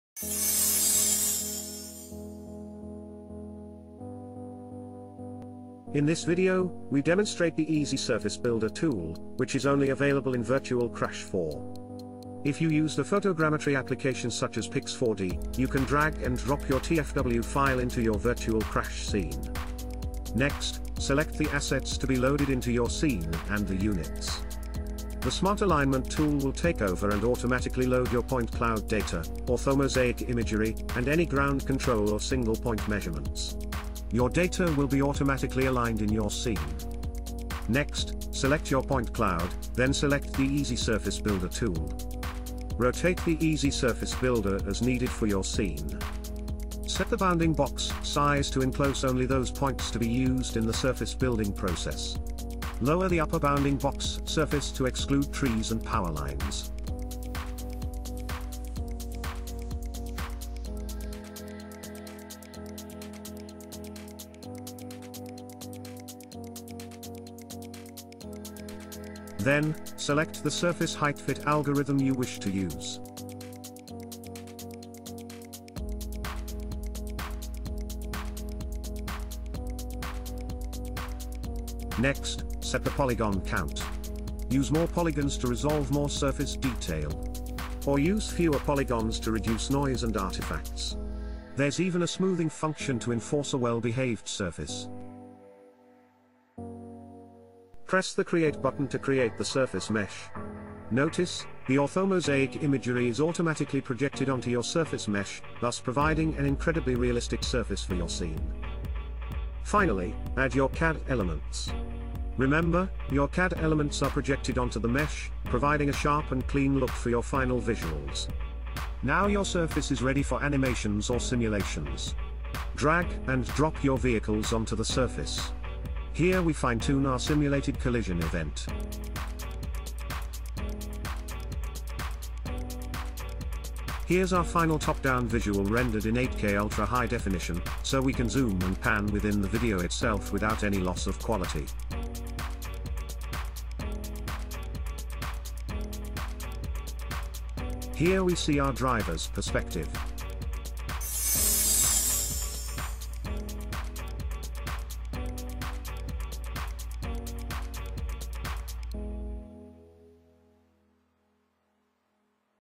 Virtual CRASH | The Easy Surface Builder
Virtual CRASH is a general purpose physics simulation tool primarily for motor vehicle accident reconstruction. With Virtual CRASH, you can simulate complex multi-vehicle simulations and render them in stunning high quality video. With Virtual CRASH 4, users can import point cloud data and build surfaces meshes from point clouds.